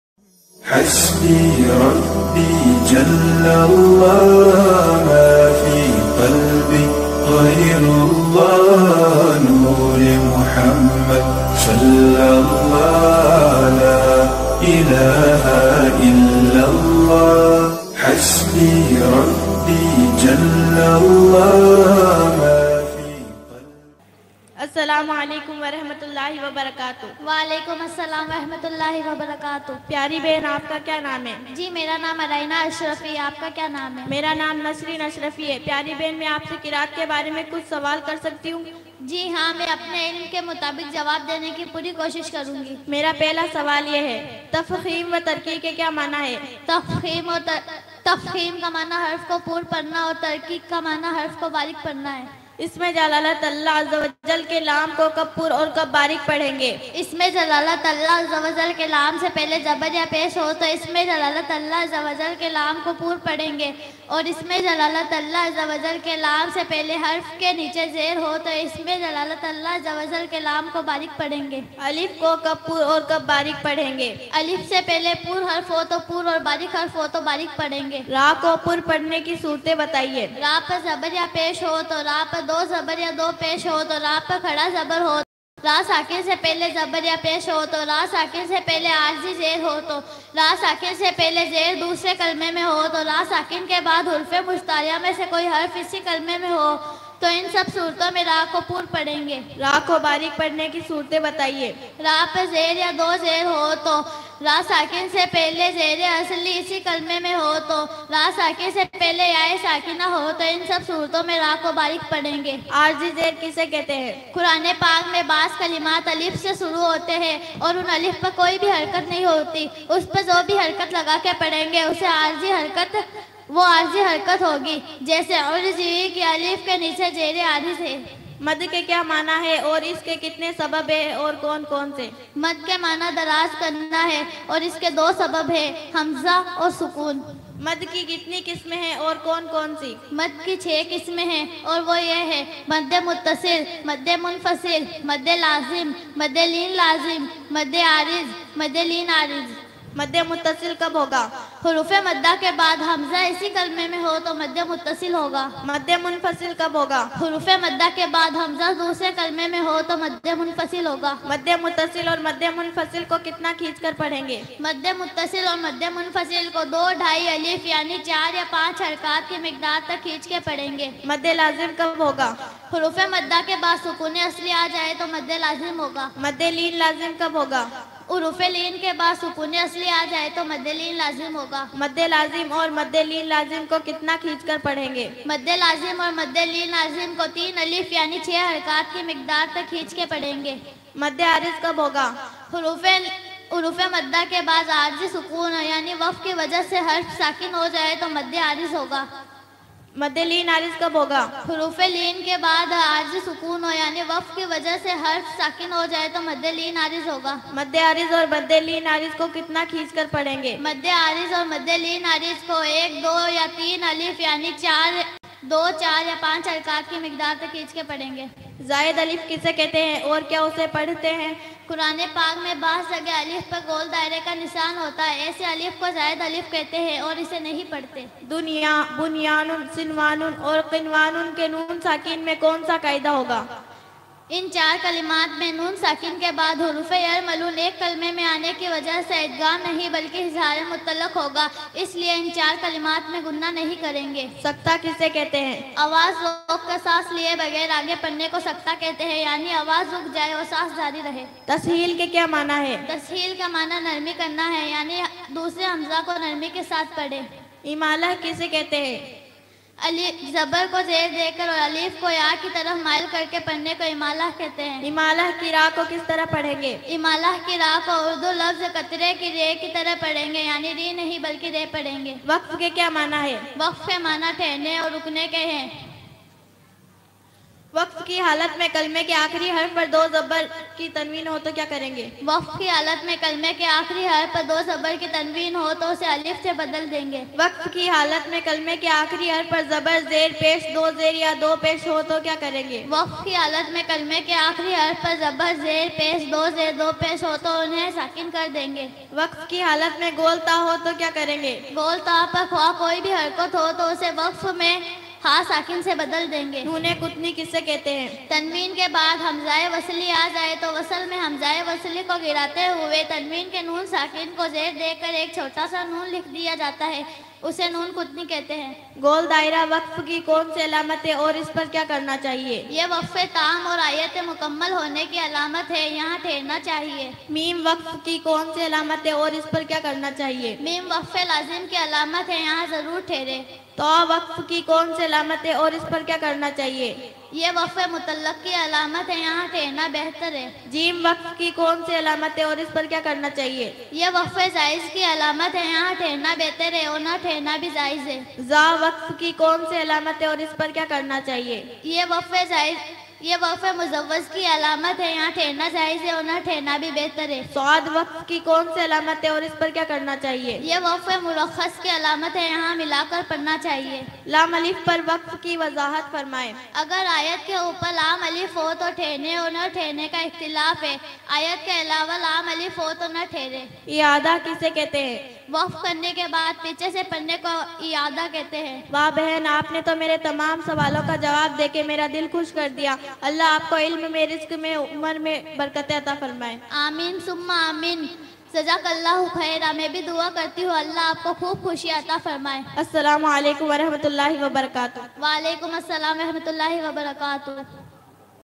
मदनी क़ायदे के सवाल जवाब | Madani Qaaide ke Sawal Jawab At Merta City | مدنی قائد کے سوال جواب
Live stream program

गौस ए आजम की विलादत का वक़िया | मौलाना हाफिज इस्माइल साहब की तकरीर  | Hafiz ismail shahb ki takreer

दर्श ए कुरान | मुहम्मद अकरम साहब उस्मानी | darsh e Quran  Mohammed Akram Shahb At Merta City